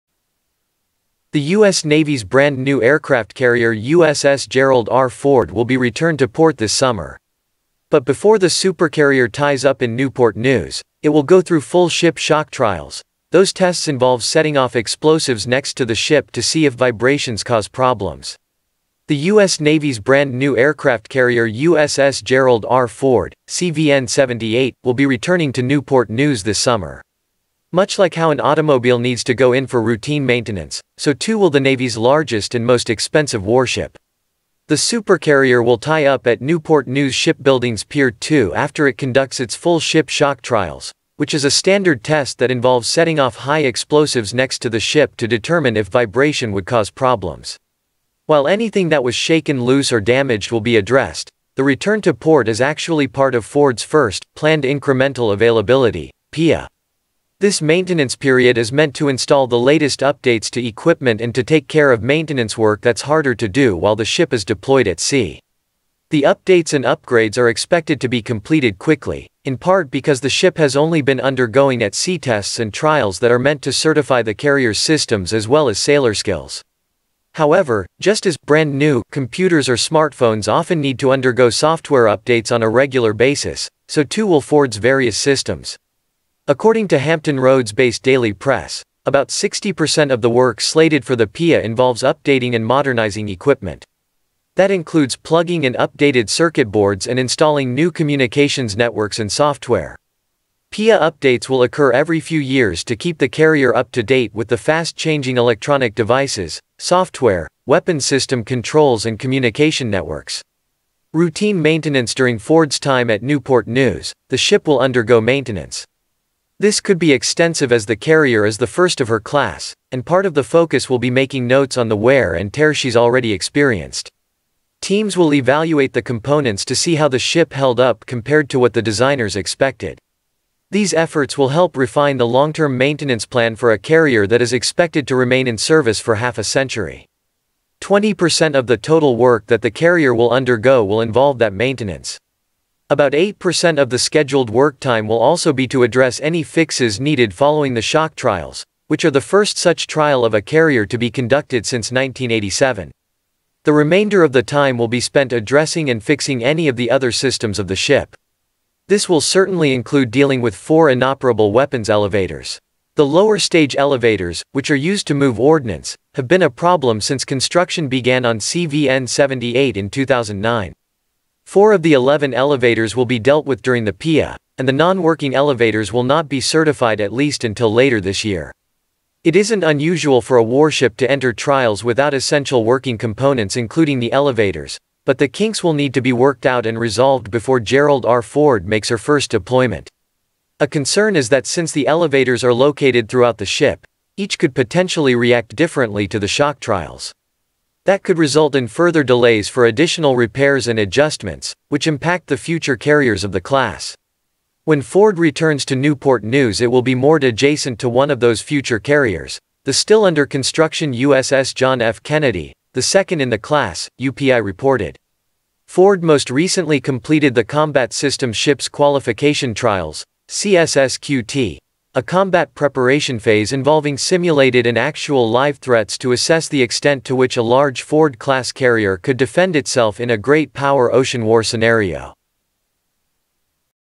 The Navy's New, Powerful Aircraft Carrier is Heading to 'Shock Trials'
The US Navy's brand-new aircraft carrier USS Gerald R. Ford will be return to port this summer.
But before the supercarrier ties up in Newport News, it will go through full-ship shock trials,
Those tests involve setting off explosives next to the ship to see if vibrations cause problems.